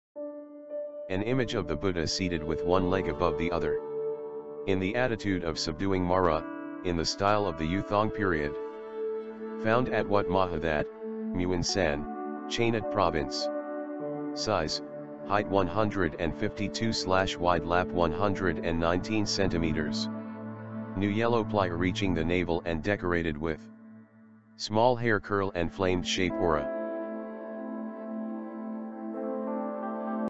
(19) An image of the Buddha seated with one leg above the other.
This video is part of a graduate project.
The purpose is to educate the Buddha in the balcony of Wat Benchamabophit Dusit Wanaram Thailand for Chinese tourists. Come to worship the Buddha at this temple.

Produced by Miss Juthamart Raknarong
4th year students in the communication art technology. Rajamangala University of Technology Rattanakosin
Faculty of Architecture and Design